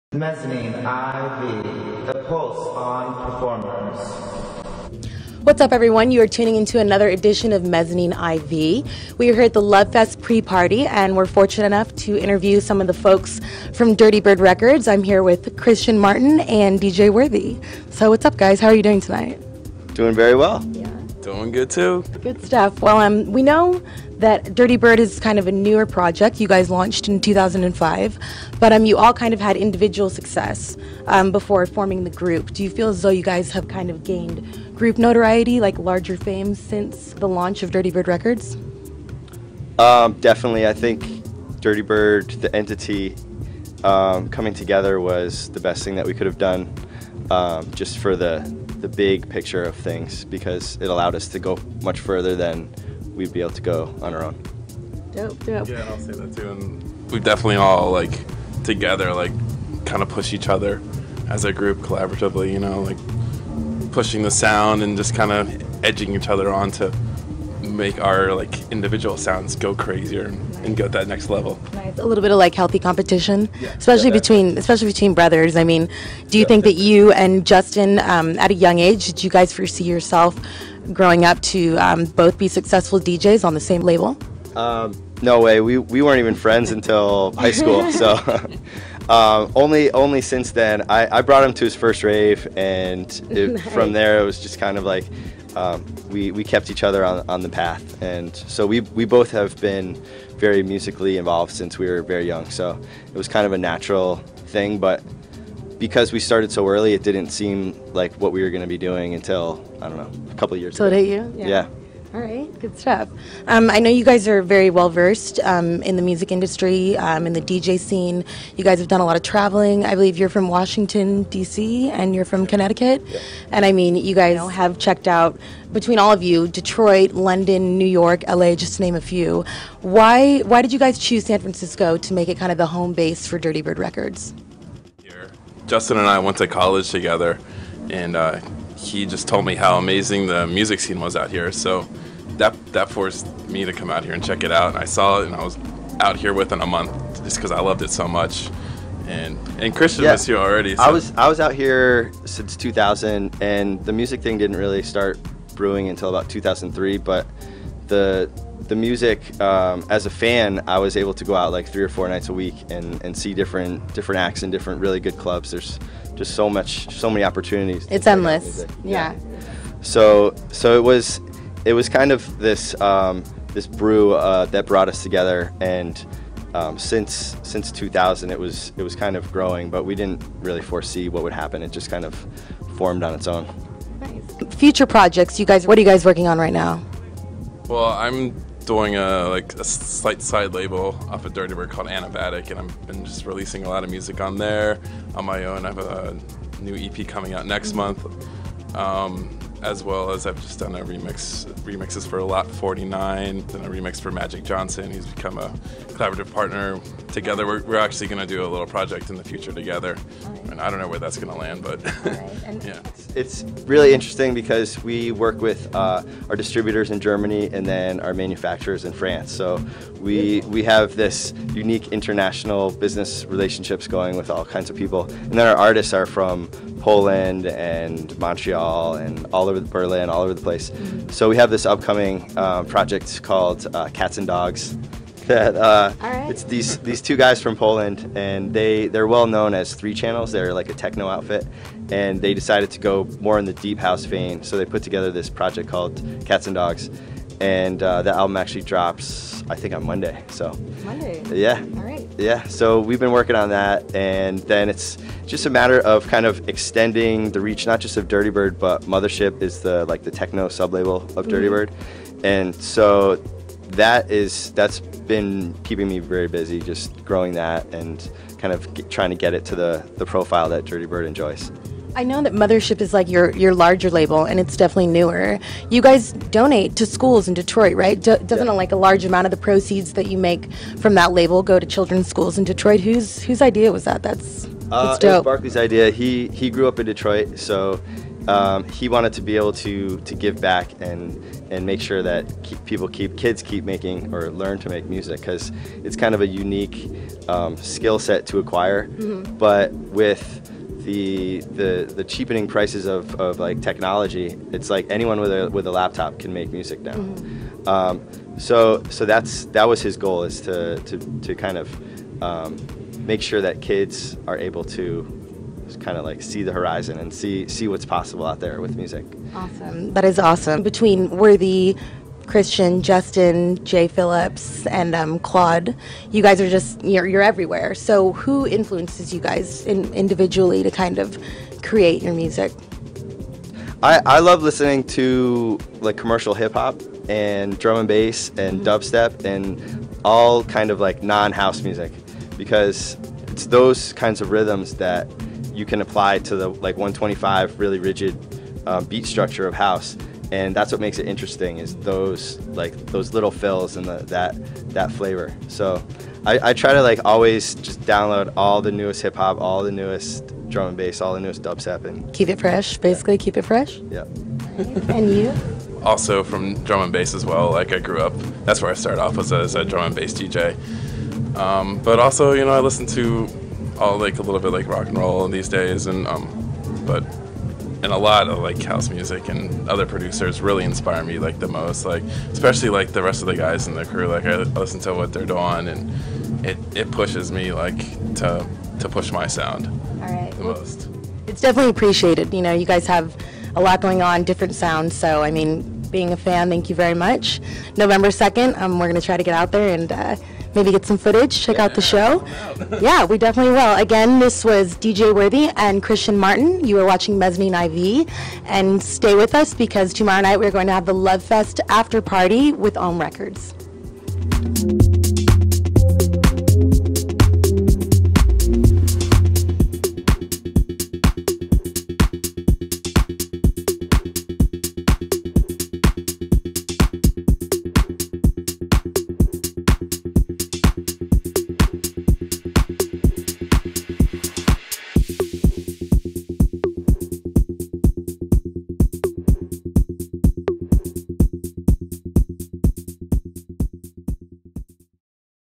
Mezzanine IV: Dirtybird Records
Al for Mezzanine San Francisco sit down with some of the Dirtybird recording artist to engage in conversation.